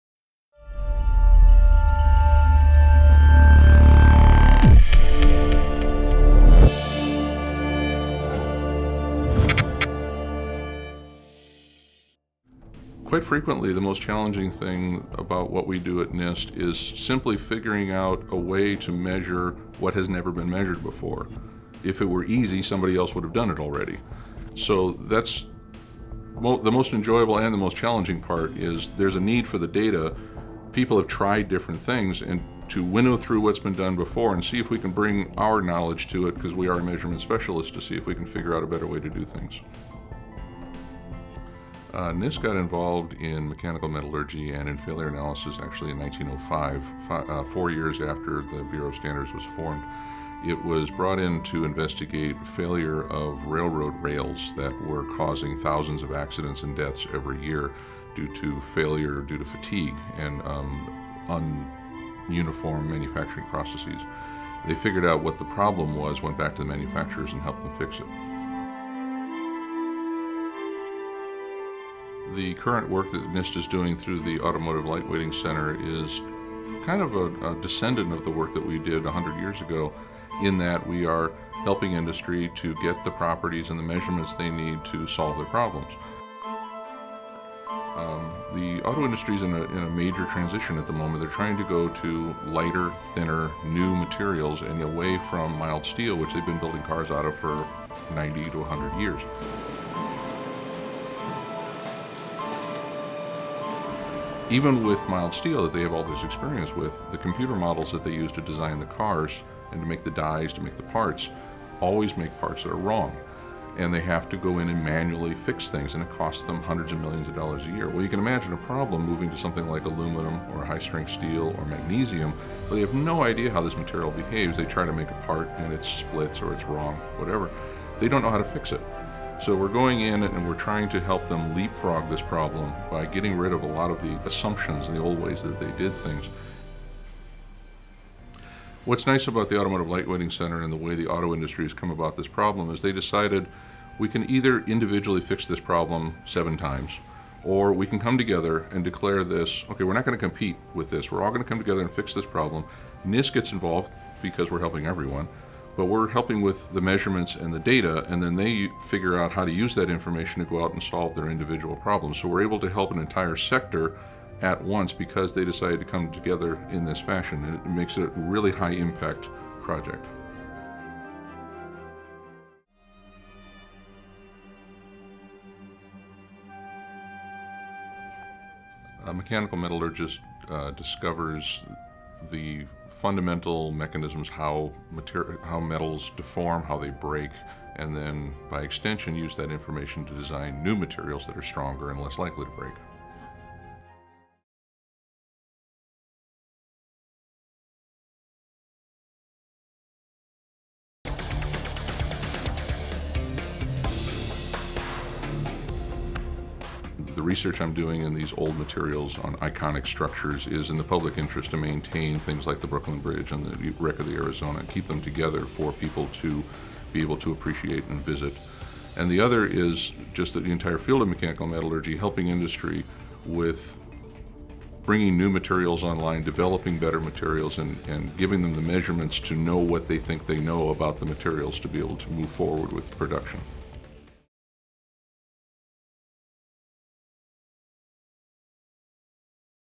NIST Unscripted: Tim Foecke
NIST mechanical metallurgist Tim Foecke discusses the science of metallurgy, why it is important for industries such as manufacturing to better understand metals and their properties, and what NIST is doing to provide that knowledge along with tests, measurements and standards.